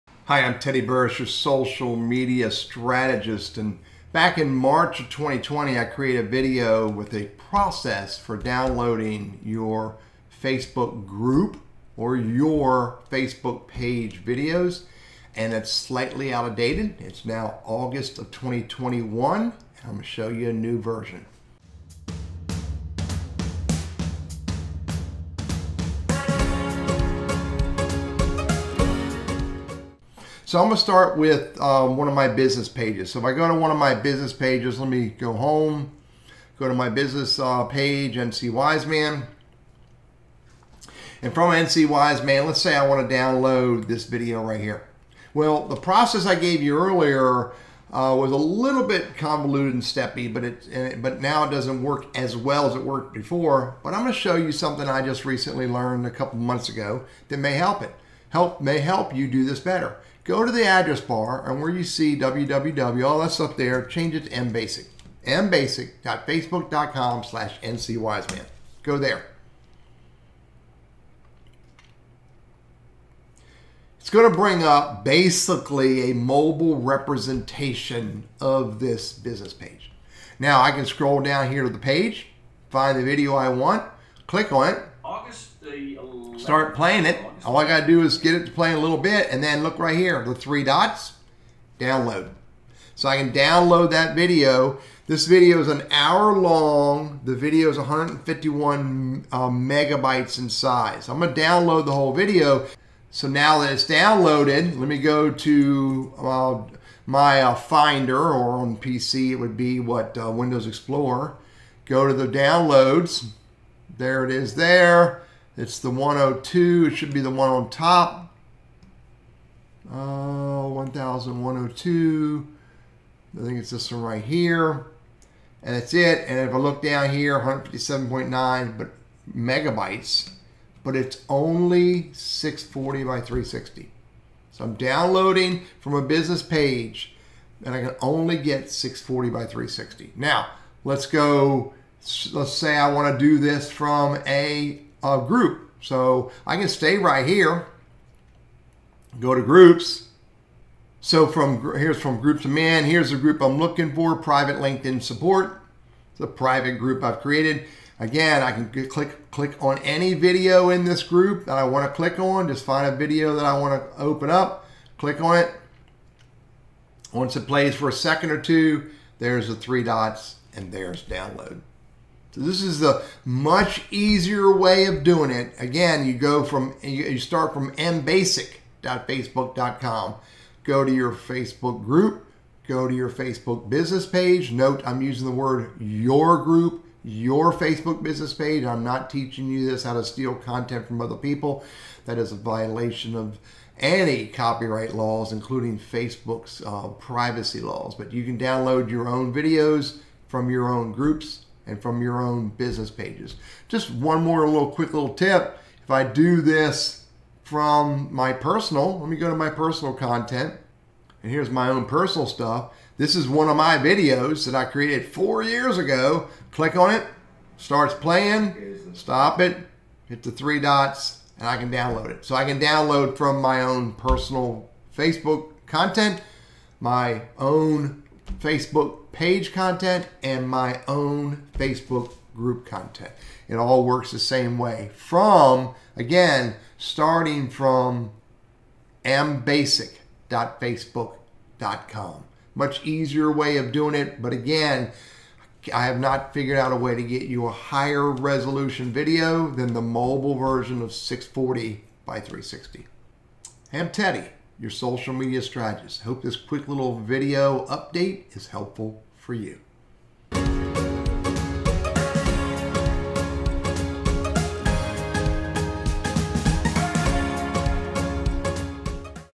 How to download a Video from your Facebook Profile, Page or Group
This video replaces a March 2020 version as the process has changed.

Go to mbasic.facebook.com to use this feature.

Teddy Burriss
LinkedIn Strategist & Trainer